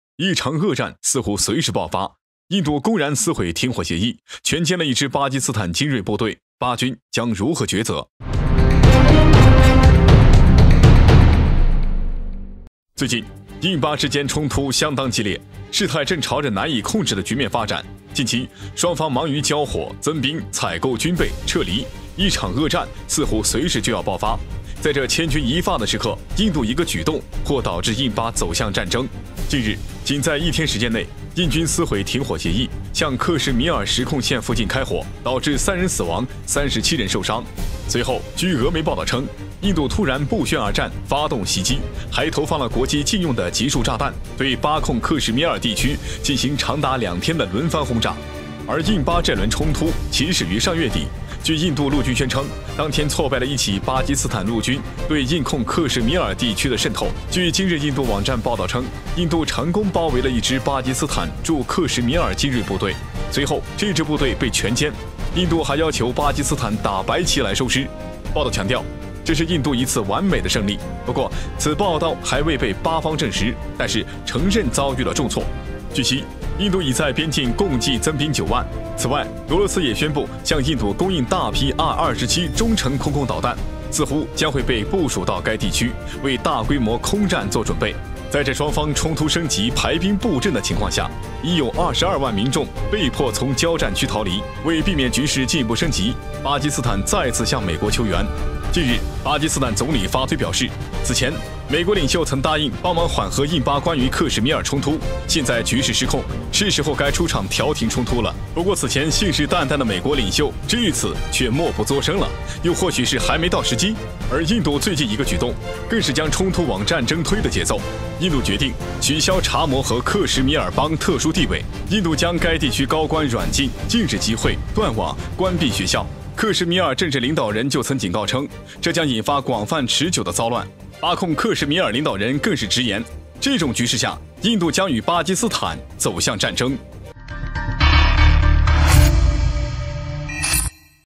印度突然不宣而战发动袭击，连续2天轮番轰炸，巴精锐部队遭全歼
近日，仅在一天时间内，印军撕毁停火协议，向克什米尔实控线附近开火，导致3人死亡，37人受伤。（晶晶） 我們的影片會遵循公平、尊重的原則，確保不會包含任何仇恨言論的元素。